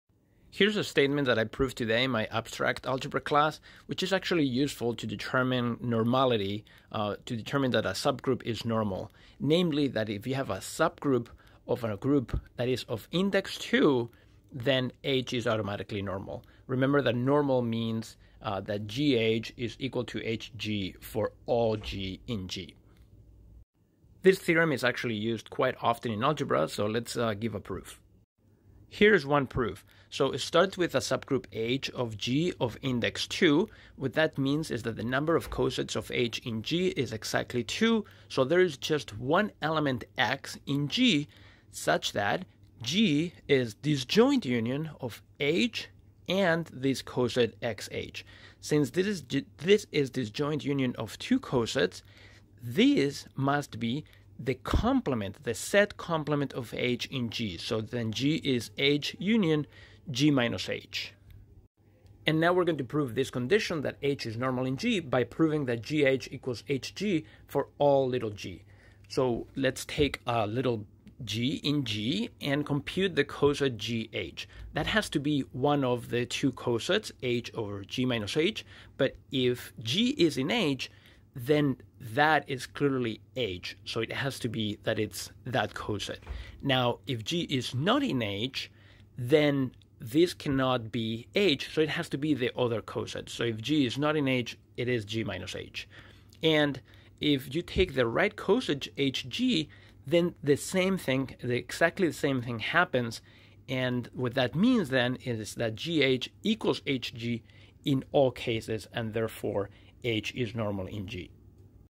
Subgroups of index 2 are normal
In this video we show a statement in group theory about normal subgroups.